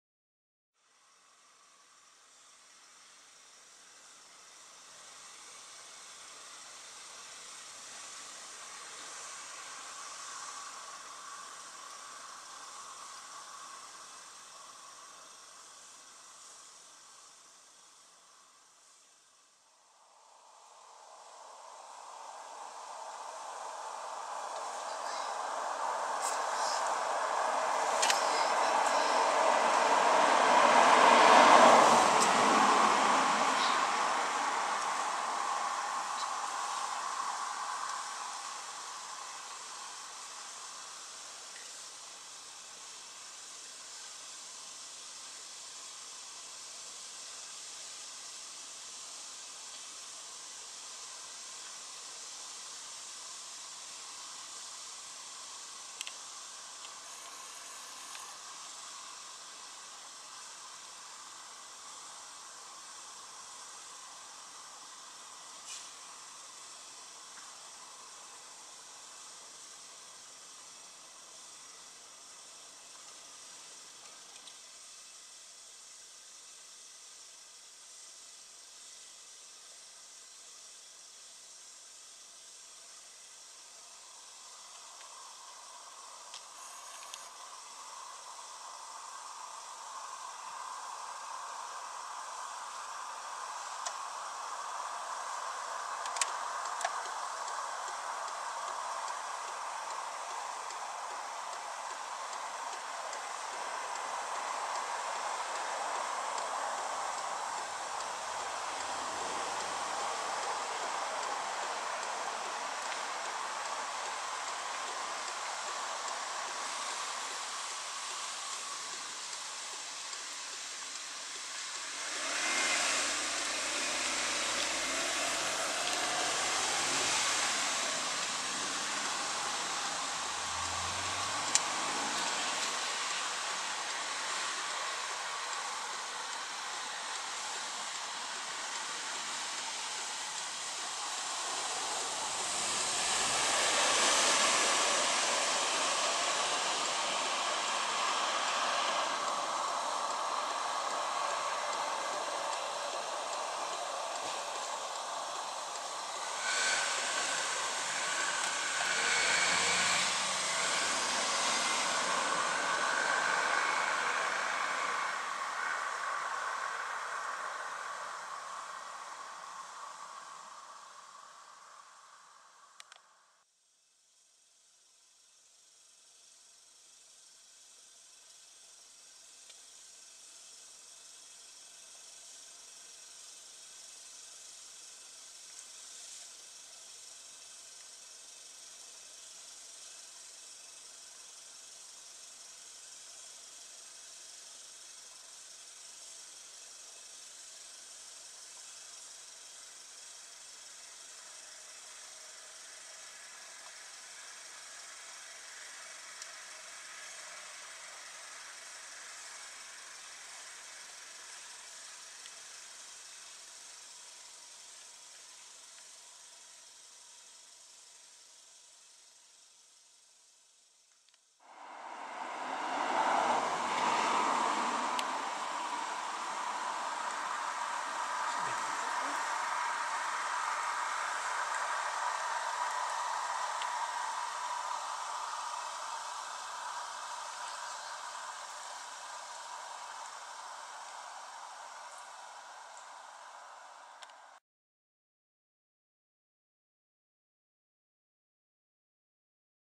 Wilde zwijnen - Sus scrofa - langs de Aardhuisweg - Kroondomein - Het Loo - The Netherlands.
Wilde zwijnen (Sus scrofa) langs de Aardhuisweg in het Kroondomein Het Loo.

(Uddel: 28 september 2013).